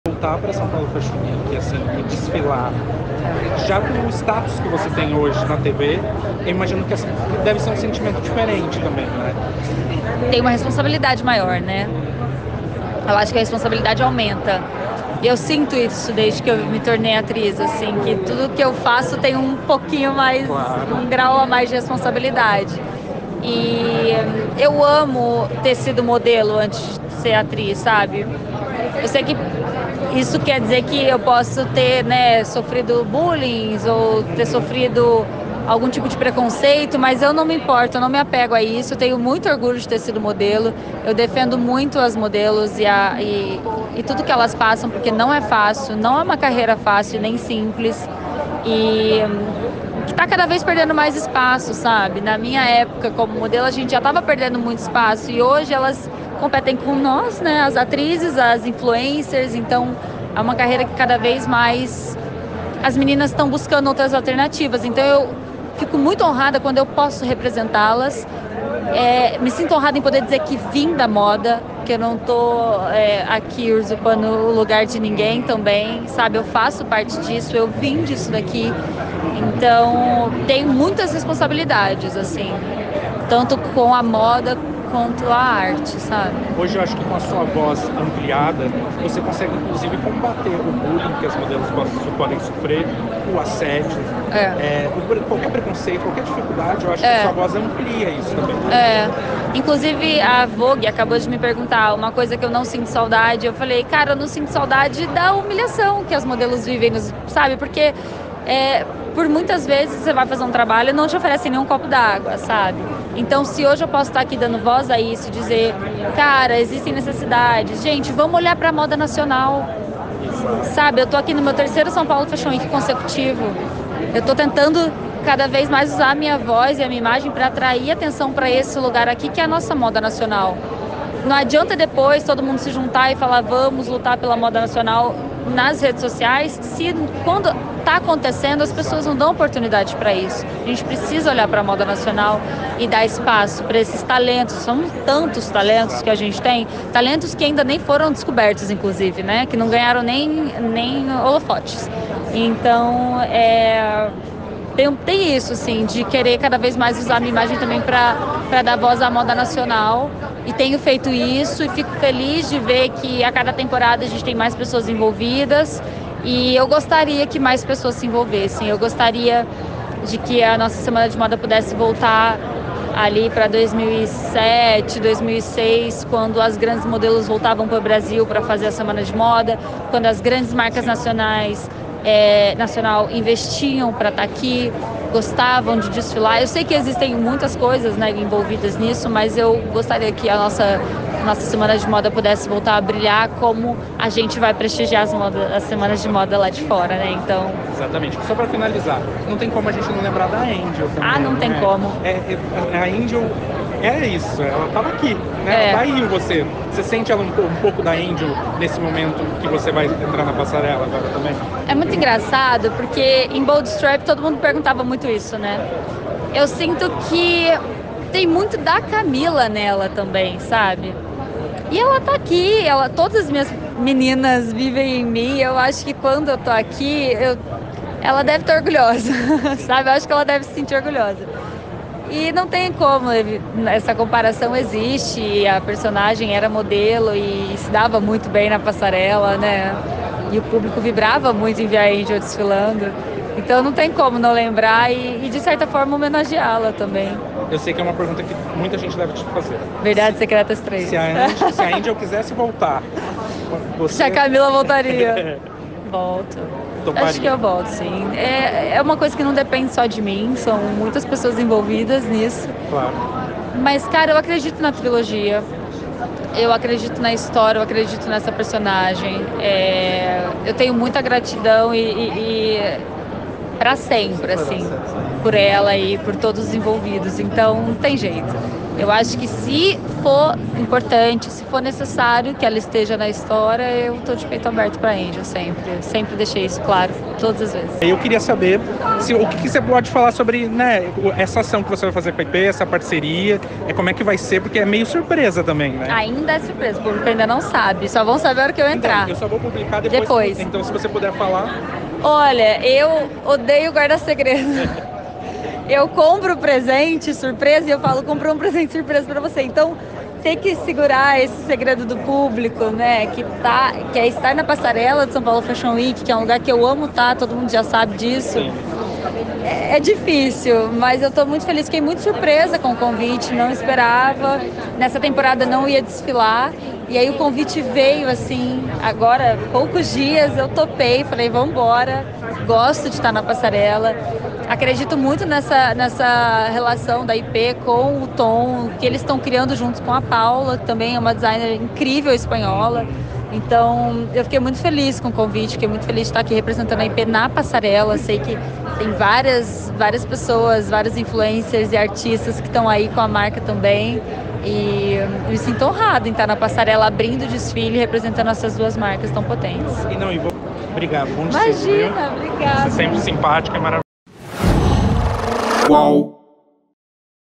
Camila Queiroz faz desabafo sobre época de modelo: 'Não sinto falta da humilhação'
Camila Queiroz abriu o jogo sobre seus tempos de modelo e lançou um alerta sobre as dificuldades da profissão e como ela quer usar sua visibilidade no mercado para trazer mais atenção a moda nacional e para as profissionais das passarelas